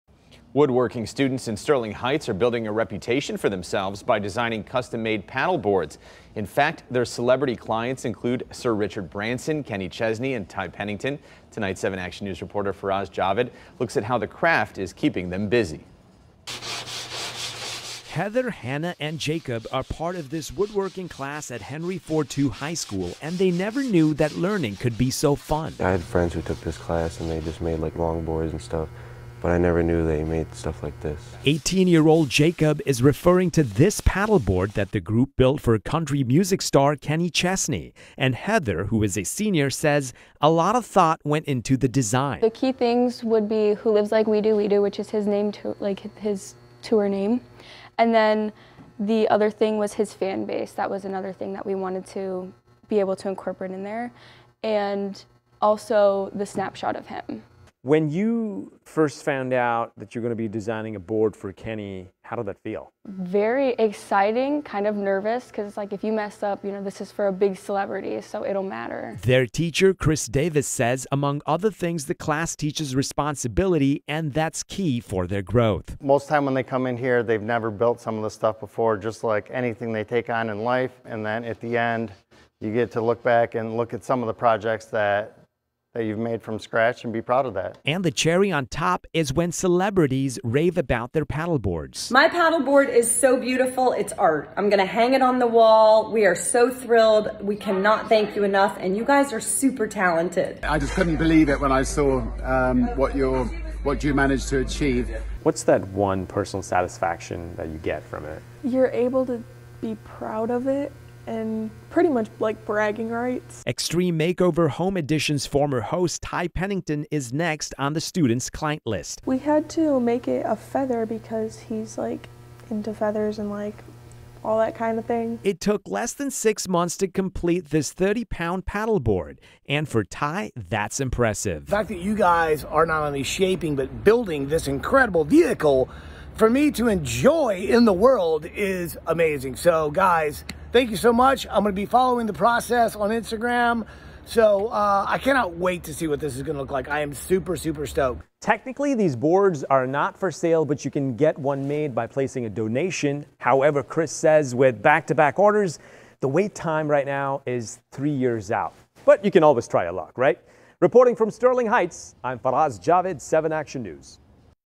Sterling Heights teens building a reputation through their woodworking classes
Henry Ford II High School students are learning how to be creative with wood. Chris Davis' woodshop classes are building paddleboards and getting the attention of celebrities. From Ty Pennington to Kenny Chesney, Henry Ford II students are getting them something to talk about.